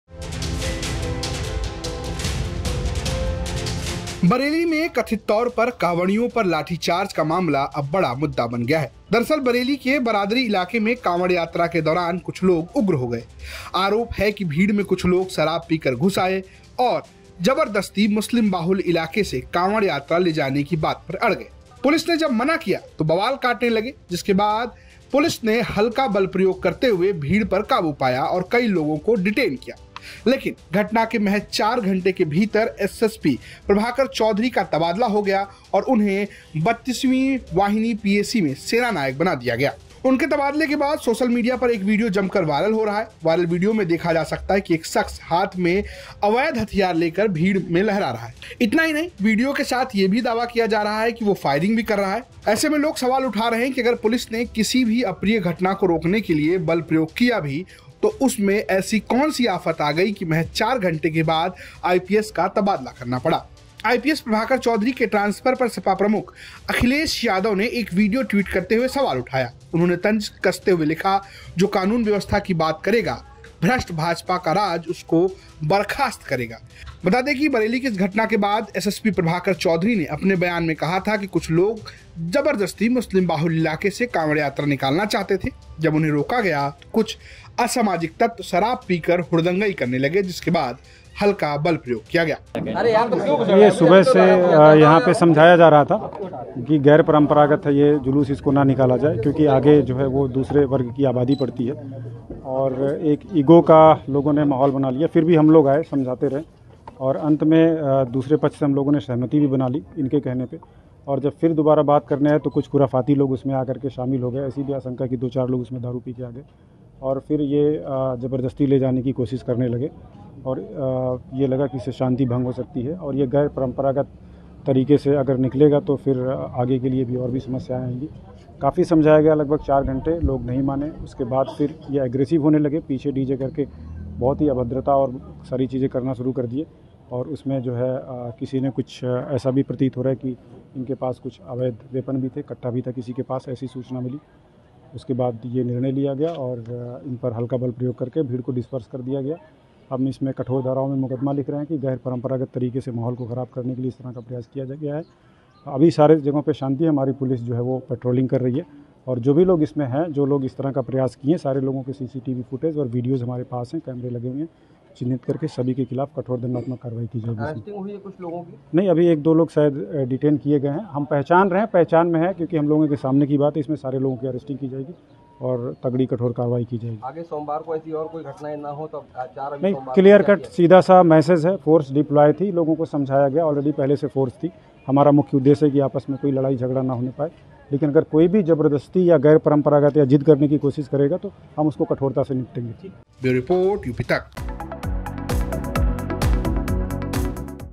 Akhilesh Yadav On Prabhakar Chaudhary: प्रभाकर चौधरी के तबादले से नाराज़ हो गए अखिलेश यादव?
Akhilesh Yadav On Prabhakar Chaudhary: बरेली में कांवड़ियों पर लाठीचार्ज के बाद एसएसपी प्रभाकर चौधरी का तबादला कर दिया गया. इसके बाद अखिलेश यादव ने ट्वीट करके सीधे बीजेपी सरकार पर हमला बोल दिया.